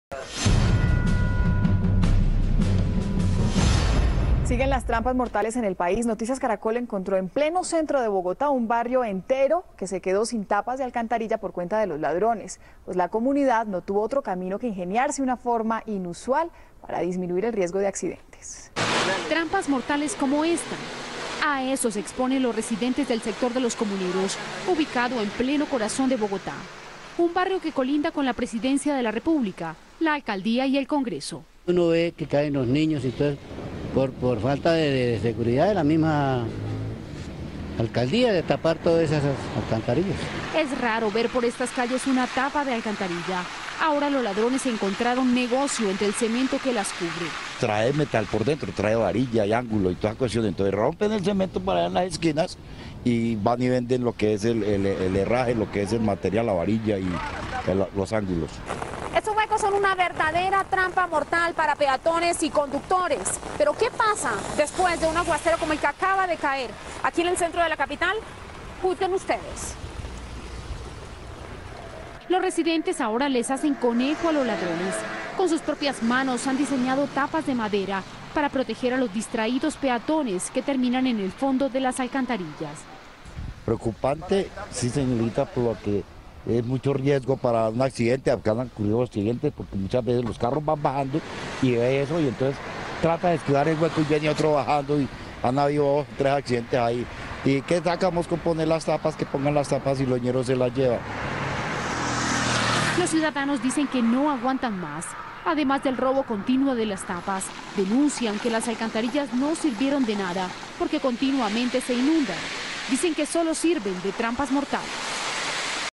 Ladrones dejaron sin tapas de alcantarilla a todo un barrio - 29 de Octubre de 2013
Habitantes de Los Comuneros, ubicado en pleno corazón de Bogotá, diseñaron cubiertas de madera para proteger a peatones.